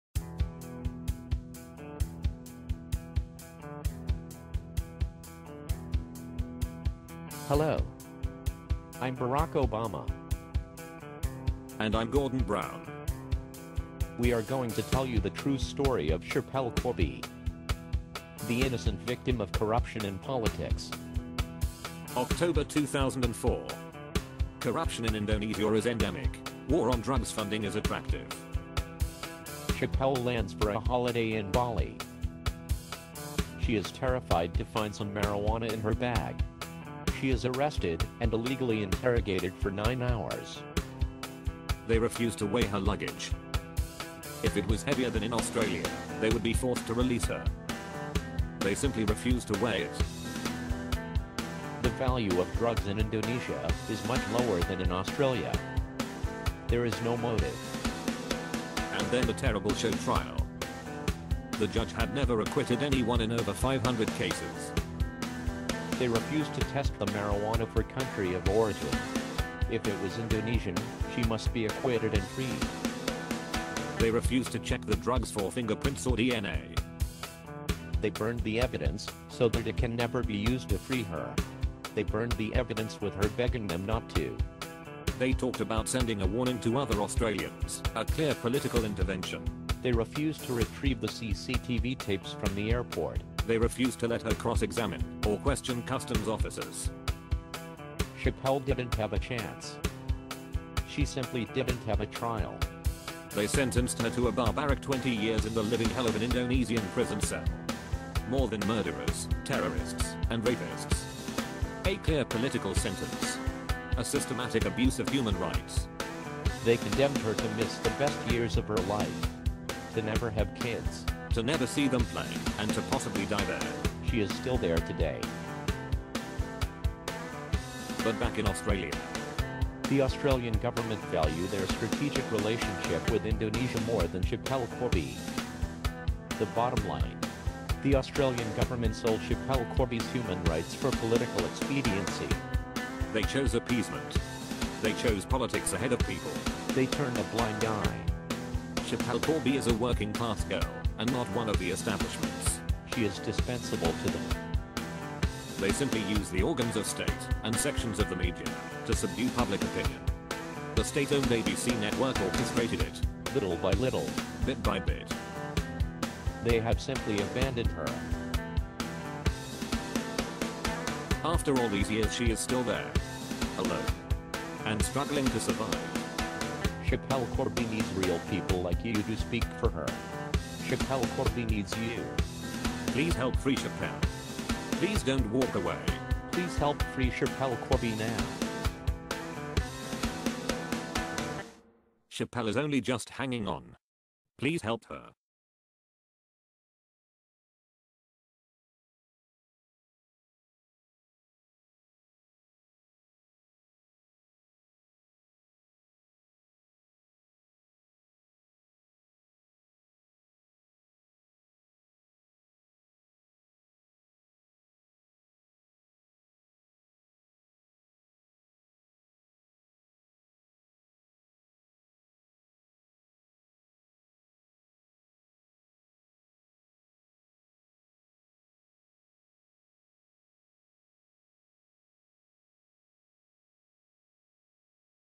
Proven facts of Schapelle Case
Just 2 of the Proven facts in the Schapelle Corby case the Australian Government did not want you to see. There are many, many more crimes that the Australian Government committed against her. All are available on the Expendable Project website.